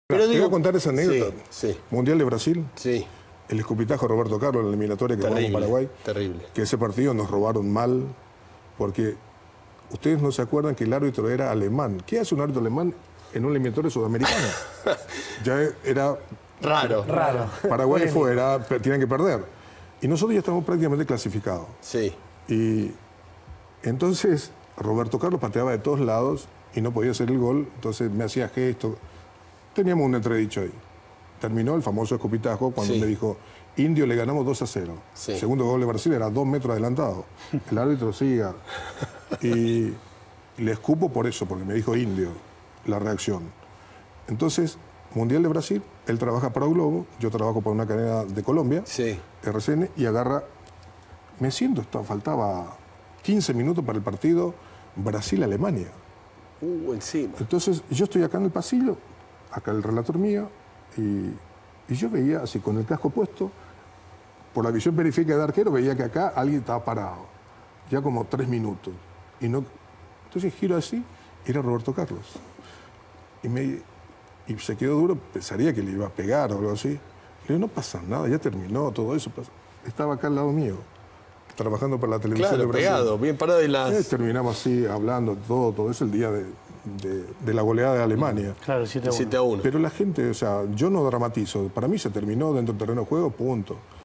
Chilavert confesó por qué escupió a Roberto Carlos, recordó su pelea
El paraguayo recordó su pelea con el defensor brasileño en las Eliminatorias y qué pasó cuando se reencontraron unos años después.